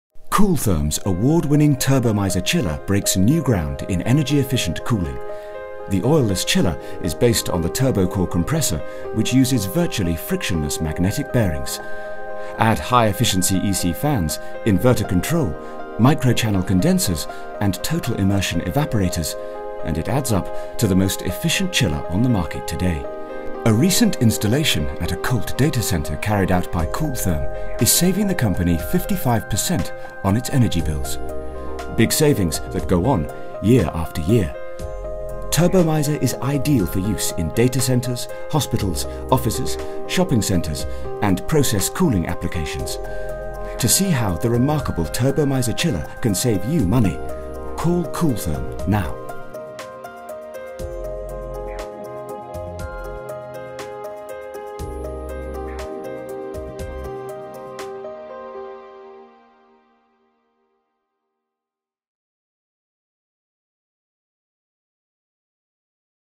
Cool-Therm's award-winning Turbomiser chiller breaks new ground in energy efficient cooling
The oil-less chiller is based on the Turbocor compressor, which uses virtually frictionless magnetic bearings.

Add high efficiency EC [said: ee-see] fans, inverter control, micro-channel condensers and total immersion evaporators - and it adds up to the most efficient chiller on the market today.

A recent installation at a Colt data centre carried out by Cool-Therm is saving the company 55 per cent on its energy bills. Big savings that go on, year after year.

Turbomiser is ideal for use in data centres, hospitals, offices, shopping centres and process cooling applications.

To see how the remarkable Turbomiser chiller can save you money, call Cool-Therm now.